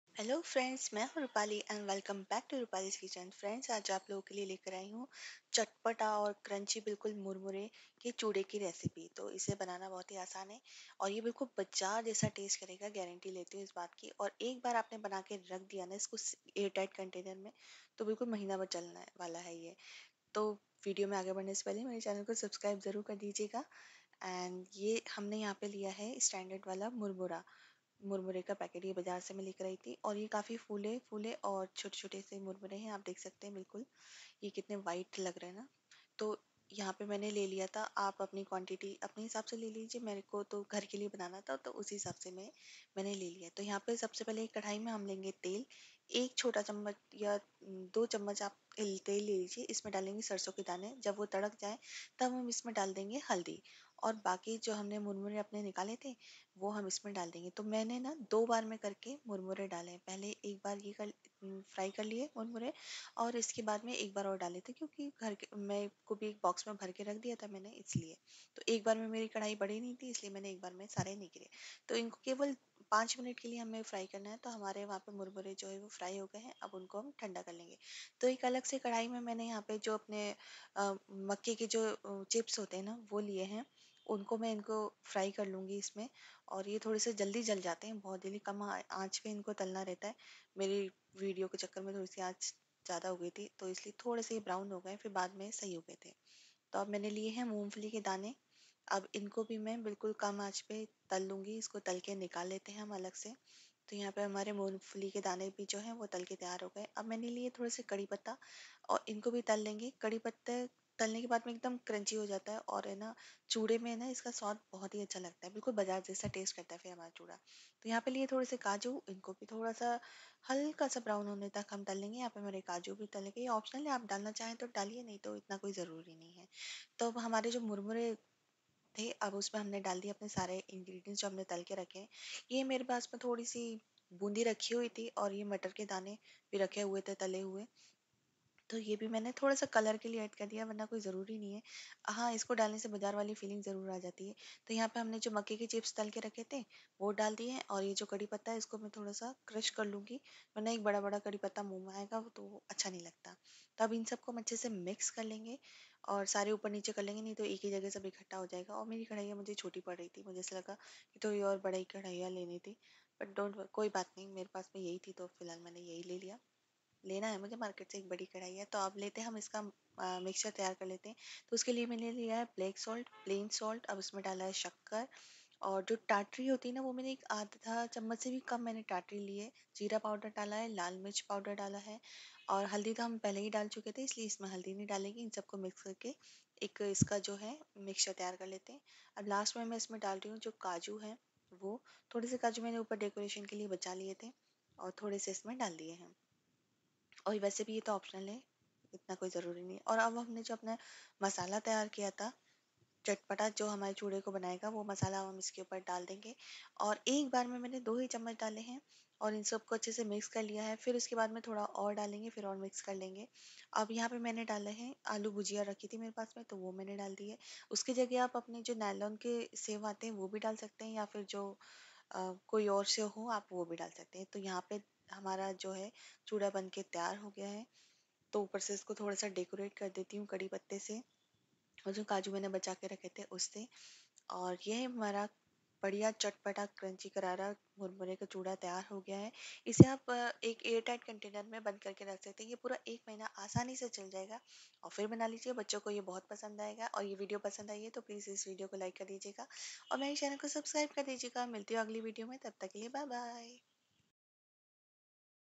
Chatpata Namkeen Murmura Recipe | Easy Chatpata Tea Time Snack Recipe | Spicy Murmura Snack
Hello Everyone,

Welcome to Roopali's Kitchen. I upload videos on daily basis.
namkeen chura
namkeen mixture
namkeen
teatime snacks